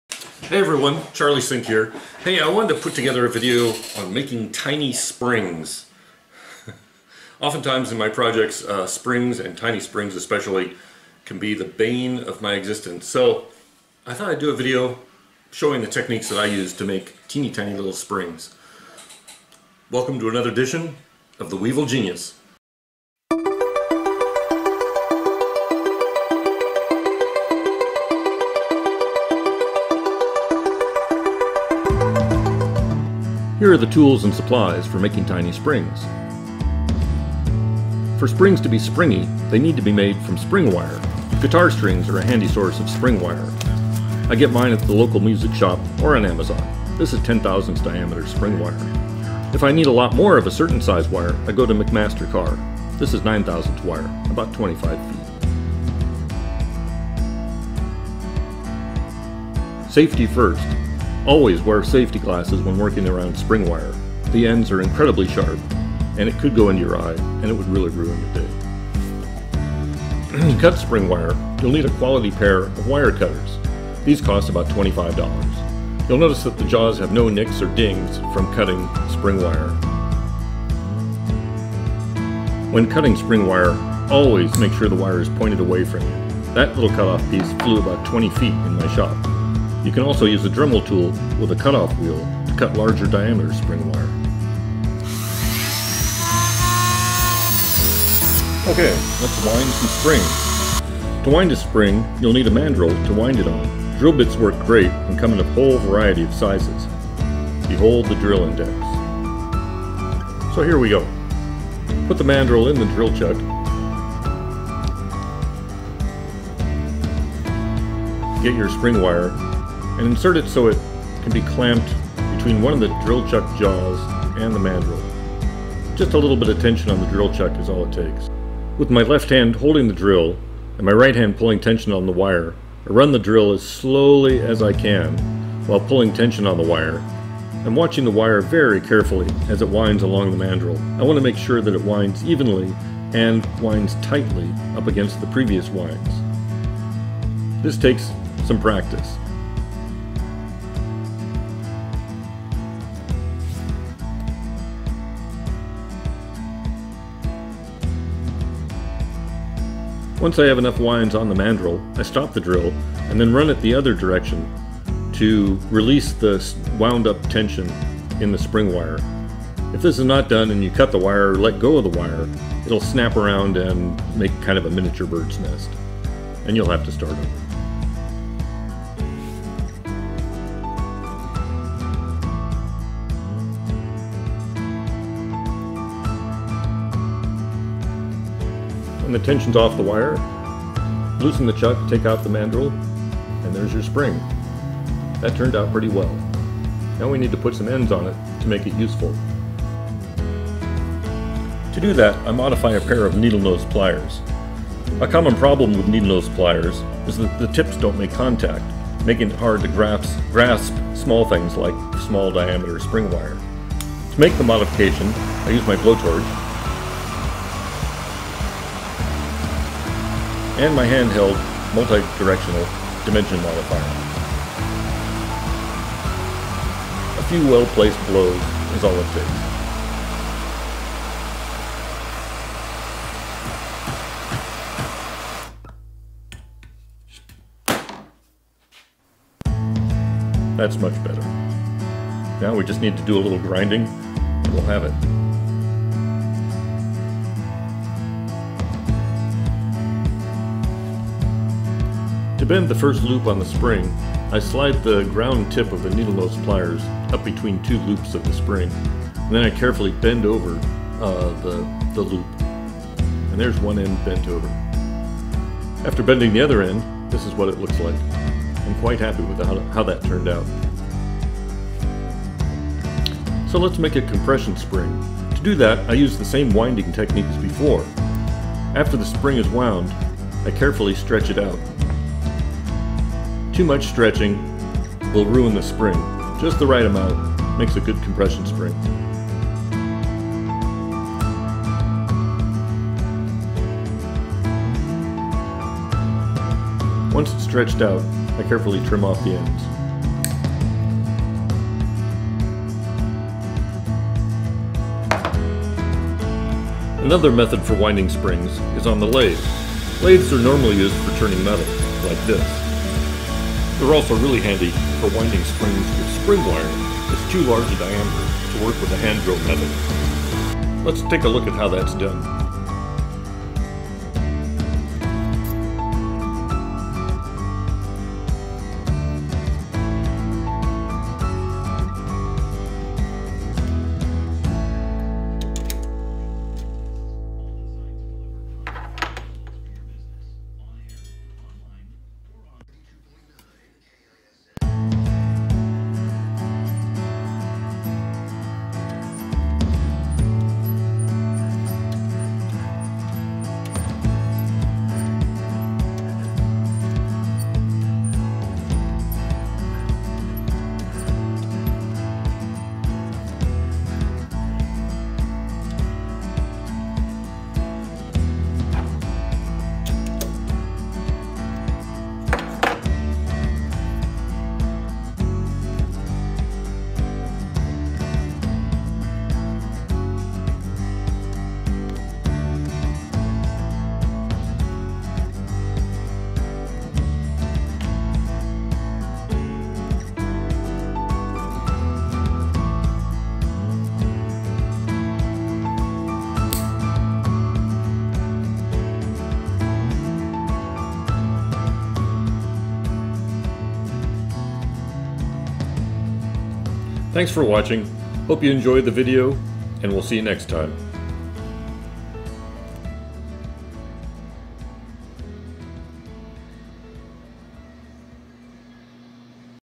How to make tiny springs
This video shows how to make tiny springs using a drill motor and a small lathe.